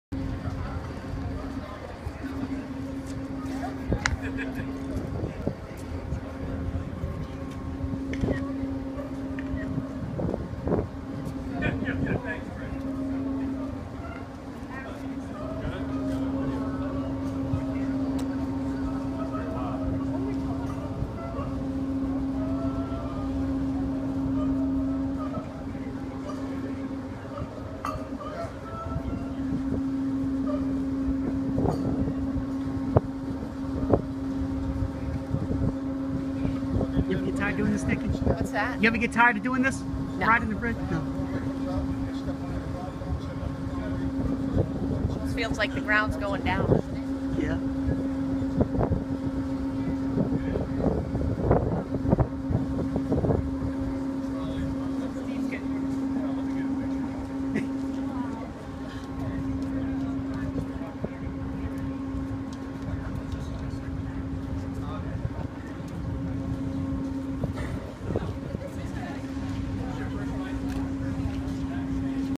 Top of the Memorial Bridge
Members of the Memorial Bridge project committee ride to the top of the bridge via the center lift span on Monday, July 28, 2014.